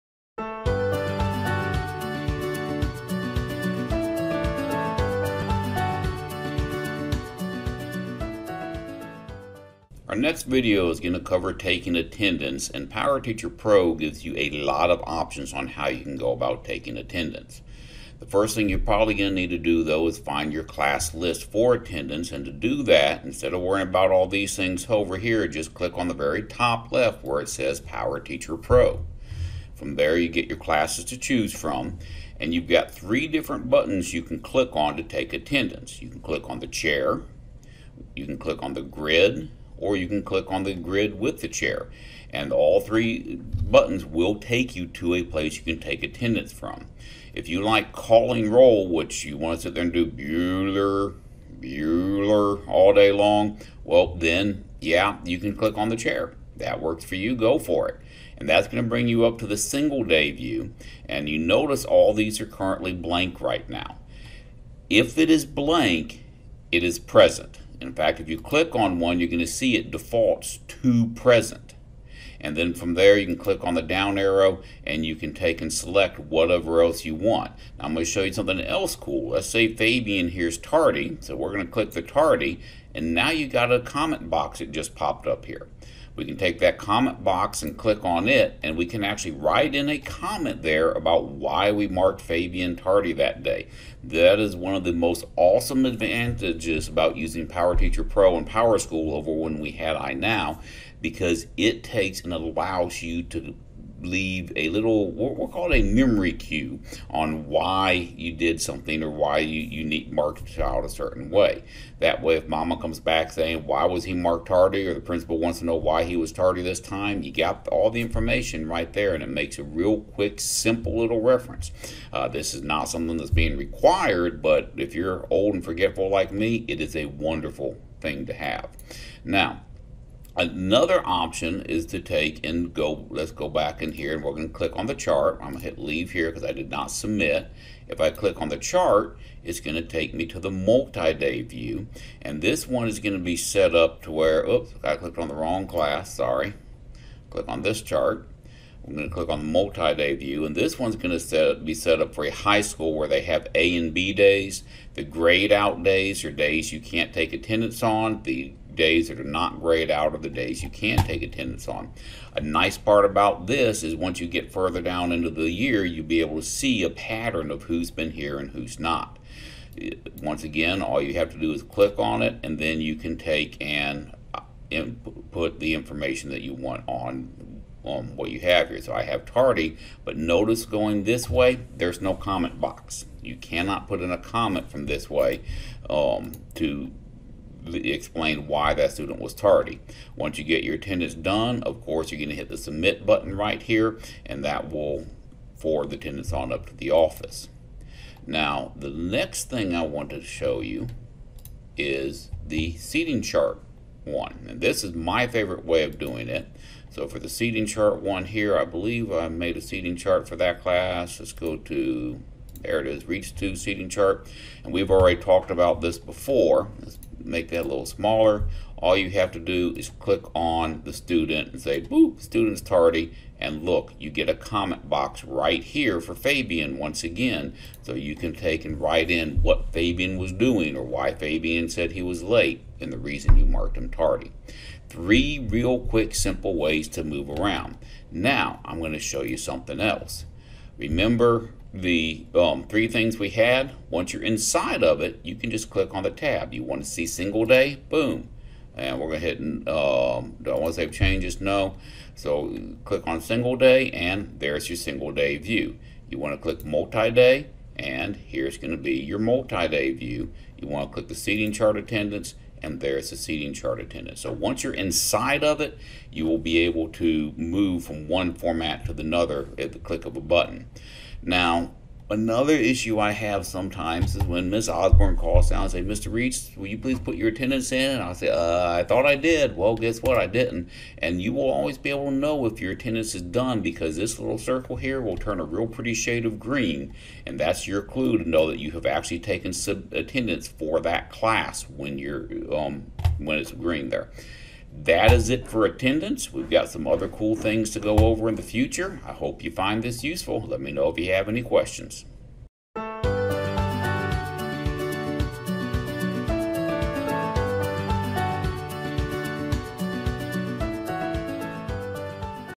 PowerSchool #5 - Taking Attendance
PowerTeacher Pro has three qreat options for taking attendance.